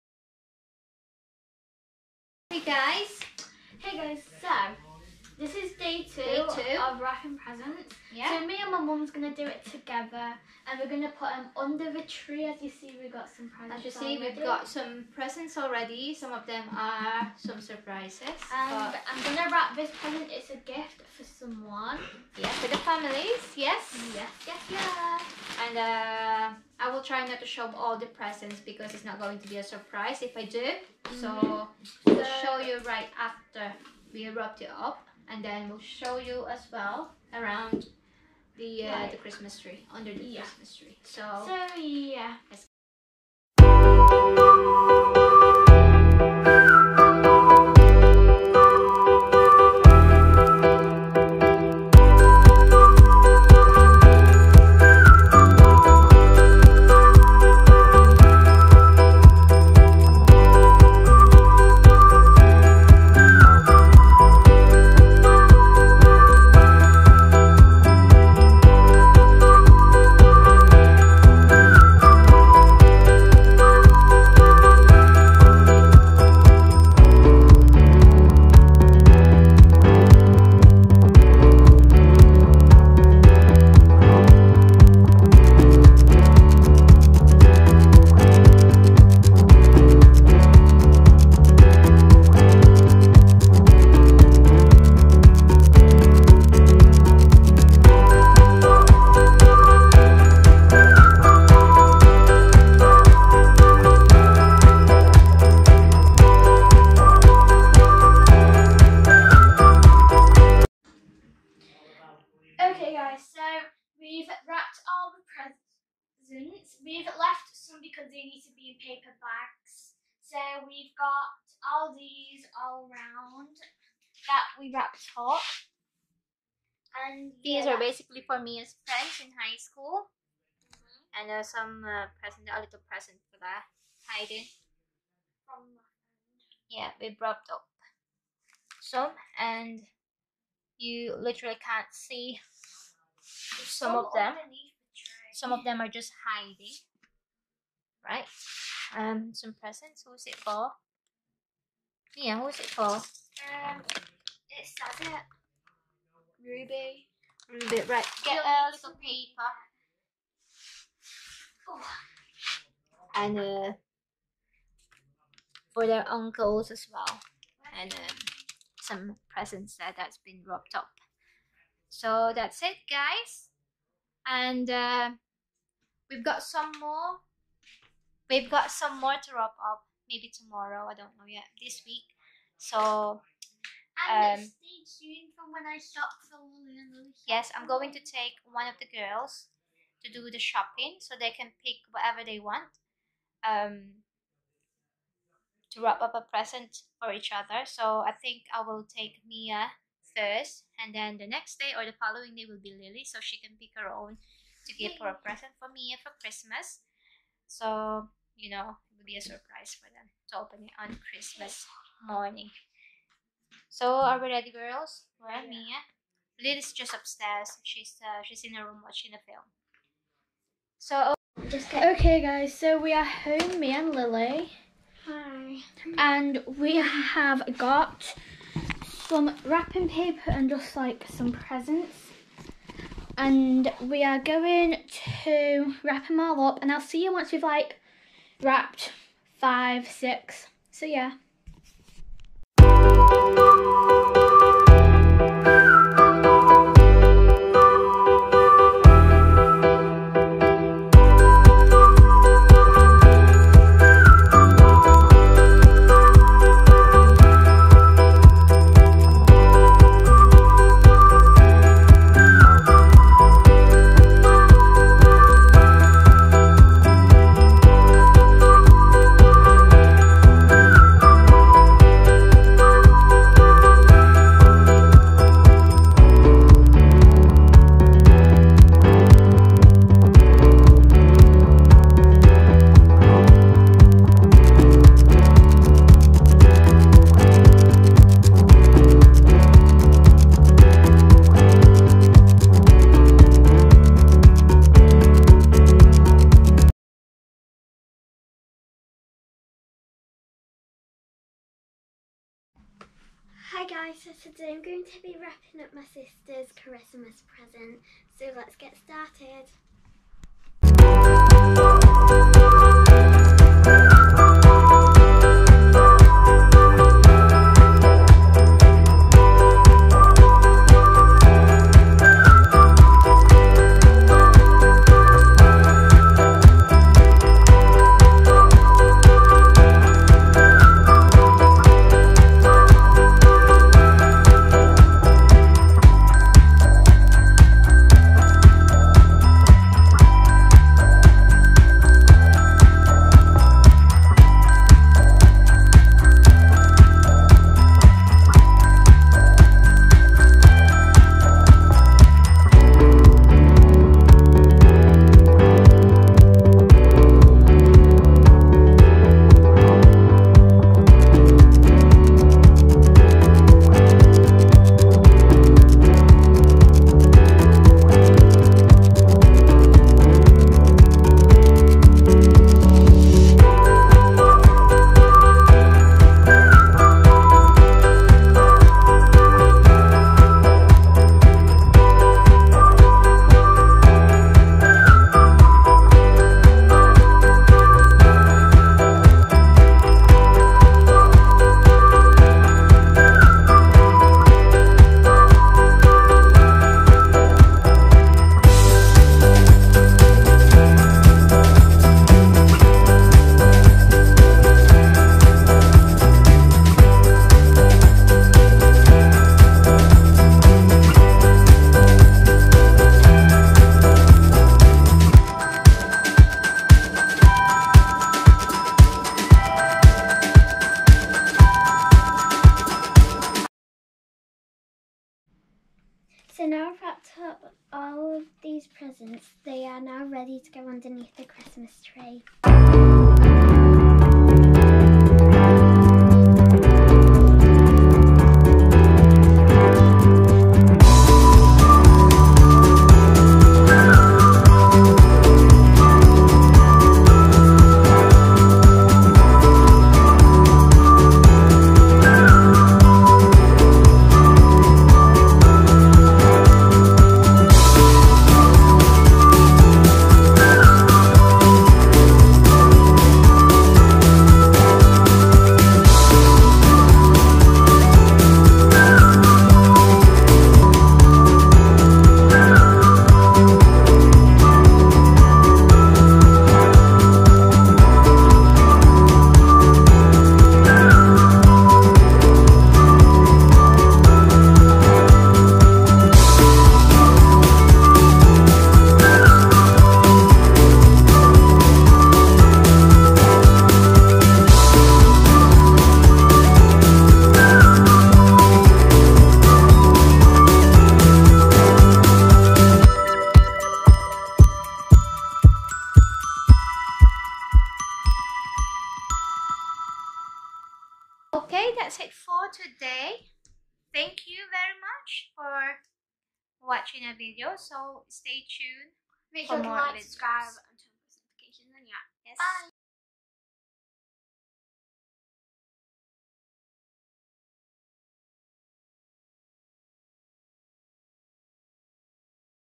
GETTING ALL READY FOR CHRISTMAS!!!
We wrapped up some of the presents to put under our christmas tree! the girls are so excited for christmas! Join us watch our premiere and we hope that you like our video.

places we went to in this video are the Following:
Tkkmaxx
B&M
Home Bargain